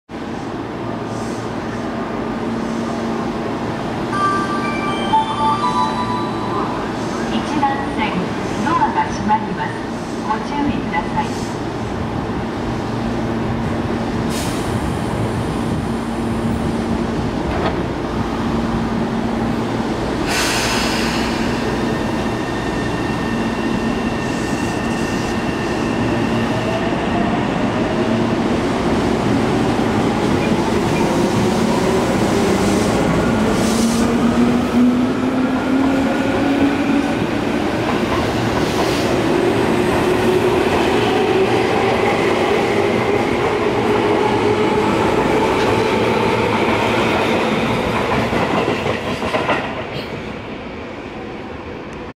東武東上線快速10000系（11006編成）朝霞台駅発車【FHD】
朝霞台駅を発車する、10000系快速森林公園行きです。
撮影：13/08/12
撮影機材：SONY NEX-7
This train is bound for Ogawamachi. This is the Tobu-Tojo line.
It is 10000 series of the Tobu Railway.